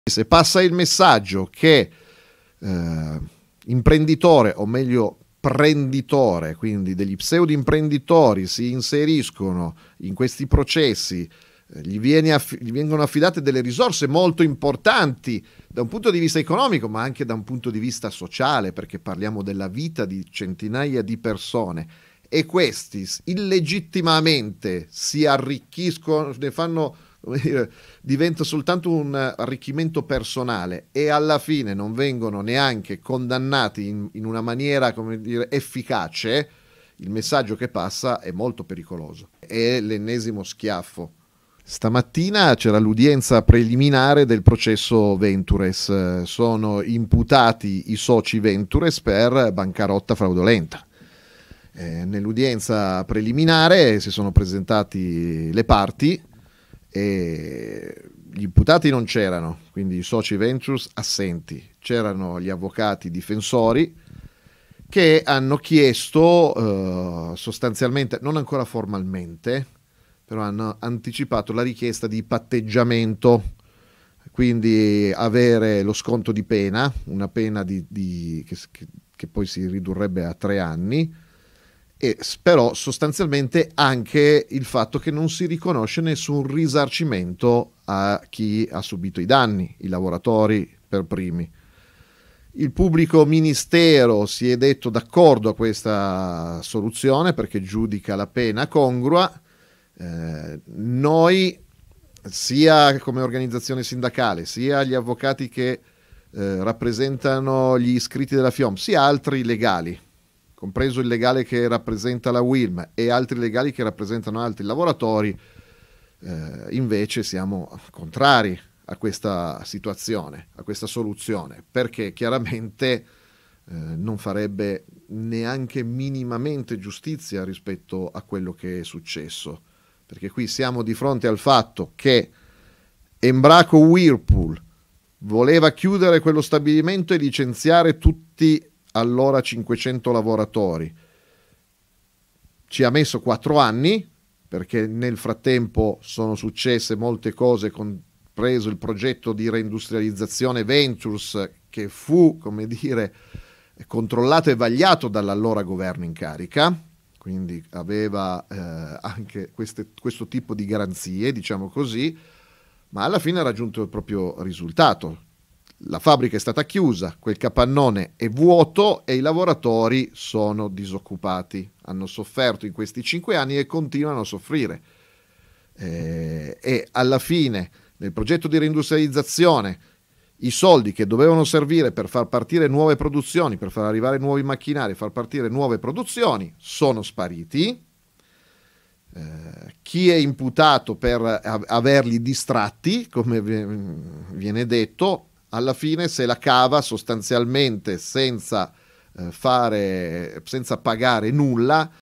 Ex Embraco, Ventures chiede patteggiamento, la Fiom: "Ennesimo schiaffo"
"Pericoloso messaggio che pseudo imprenditori possono cavarsela senza risarcimenti"

 (Torino). "Se passa il messaggio che degli pseudo imprenditori si inseriscono in questi processi con risorse importanti e questi illegittimamente si arricchiscono e non vengono neanche condannati in modo efficace è molto pericoloso. È l'ennesimo schiaffo - così Ugo Bolognesi della Fiom Torino, che questa mattina era in Tribunale per conto dei lavoratori Ex Embraco, nell'udienza preliminare contro Ventures - Stamattina c'era l'udienza preliminare contro la Ventures, imputati per bancarotta fraudolenta. Si sono presentati le parti, i soci Ventures erano assenti ma gli avvocati difensori hanno chiesto il patteggiamento per avere uno sconto di pena che si ridurrebbe a tre anni. Non si riconosce nessun risarcimento a chi ha subito i danni, lavoratori per primi. Il pm si è detto d'accordo perché giudica la pena congrua. Noi siamo contrari perché non farebbe minimamente giustizia". (Sara Iacomussi/alanews)

se passa il messaggio che imprenditore o meglio prenditore quindi degli pseudo di imprenditori si inseriscono in questi processi di vieni a chi vengono affidate delle risorse molto importanti Dal punto di vista economico ma anche da un punto di vista sociale perché parliamo della vita di centinaia di persone e questi illegittimamente si arricchiscono le fanno vedere divento soltanto un arricchimento personale e alla fine non vengono neanche condannati in una maniera come piace il messaggio che passa è molto pericoloso è l'ennesimo schiaffo Stamattina c'era l'udienza preliminare del processo venturas sono imputati i soci venture spare bancarotta fraudolenta nel udienza preliminare Si sono presentati le parti e gli imputati non c'erano  soci ventus assenti c'erano gli avvocati difensori che hanno chiesto sostanzialmente non ancora formalmente però hanno anticipato la richiesta di patteggiamento quindi avere lo sconto di pena una pena di che poi si ridurrebbe a 3 anni e spero sostanzialmente anche il fatto che non si riconosce nessun risarcimento a chi ha subito i danni i lavoratori per primi il pubblico ministero si è detto d'accordo a questa soluzione perché giudica la pena congrua noi sia come organizzazione sindacale sia gli avvocati che rappresentano gli iscritti della Fiom si altri legali compreso il legale che rappresenta la Wind e altri legali che rappresentano altri lavoratori invece siamo contrari a questa sì  a questa soluzione perché chiaramente non farebbe neanche minimamente giustizia rispetto a quello che è successo Perché qui Siamo di fronte al fatto che embraco Whirlpool voleva chiudere quello stabilimento e licenziare tutti Allora 500 lavoratori  ci ha messo 4 anni perché nel frattempo sono successe molte cose con preso il progetto di reindustrializzazione ventus che fu come dire controllato e vagliato dall'allora governo in carica quindi aveva anche queste questo tipo di garanzie diciamo così ma alla fine ha raggiunto il proprio risultato la fabbrica è stata chiusa quel capannone è vuoto e i lavoratori sono disoccupati hanno sofferto in questi cinque anni e continuano a soffrire e alla fine il progetto di reindustrializzazione i soldi che dovevano servire per far partire nuove produzioni per far arrivare nuovi macchinari far partire nuove produzioni sono spariti Chi è imputato per averli distratti Come viene detto alla fine se la cava sostanzialmente senza fare senza pagare nulla